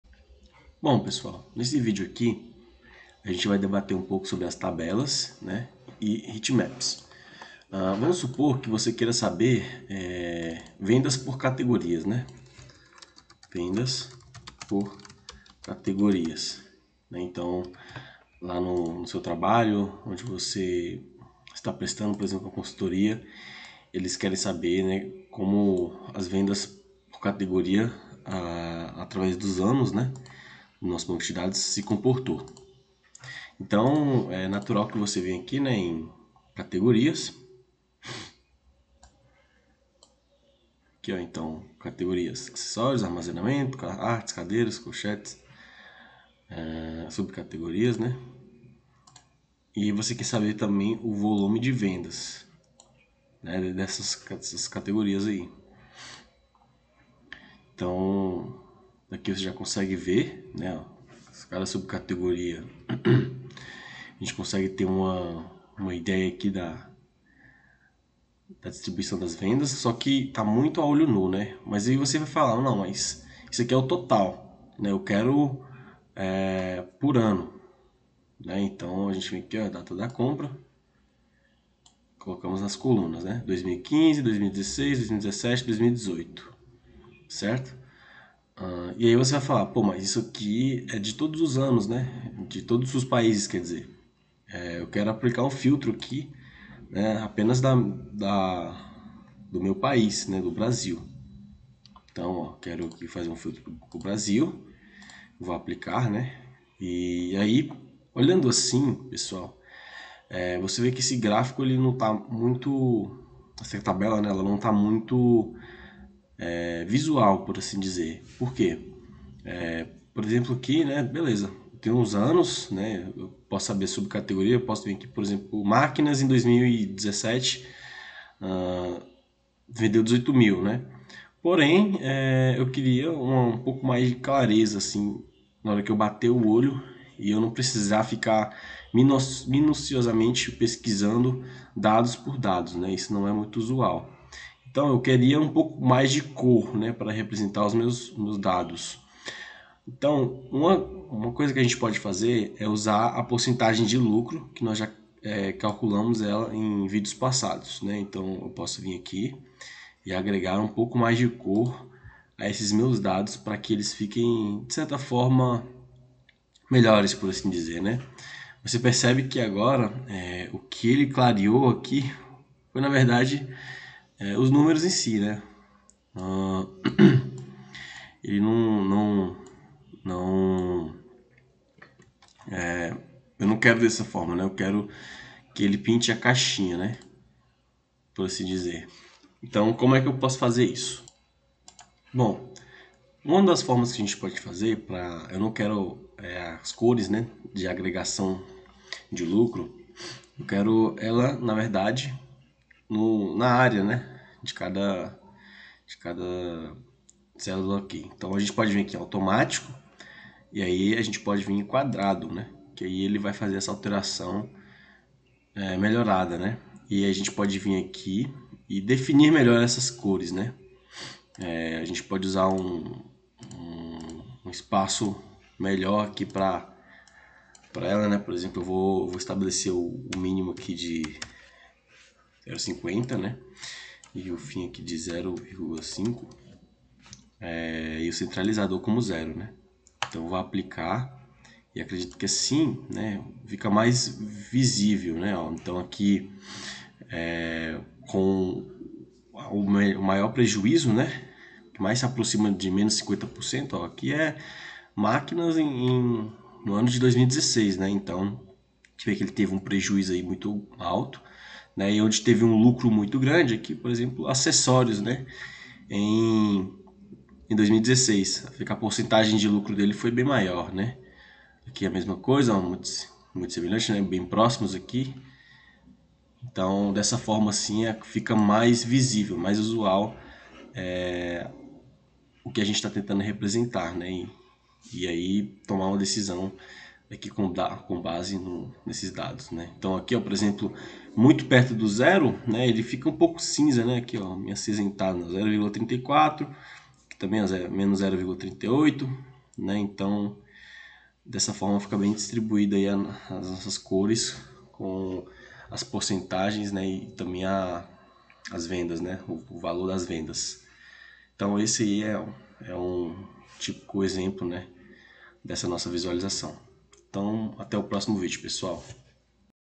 VÍDEO 12 - TABELAS E HEATMAPS NO TABLEAU
Fala pessoal!

Nesse vídeo te mostro como uma tabela pode ser mais do que um simples emaranhado de dados, elas podem trazer uma visualização mais eficiente, facilitando na hora da tomada de decisão com os diversos recursos que o Tableau pode oferecer nas suas criações.

Deixe o seu like, comenta e compartilha!